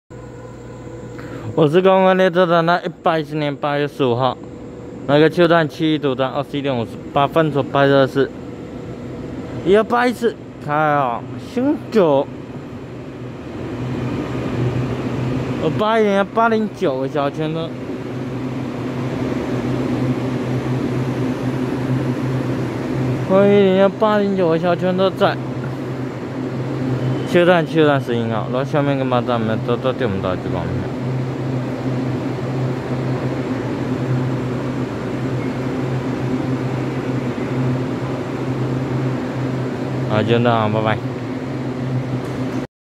1281次基隆發車經由西部幹線開往新竹的EMU810+EMU809微笑號區間車
110/8/15七堵站21:58分拍攝1281次基隆發車經由西部幹線開往新竹的EMU810+EMU809微笑號區間車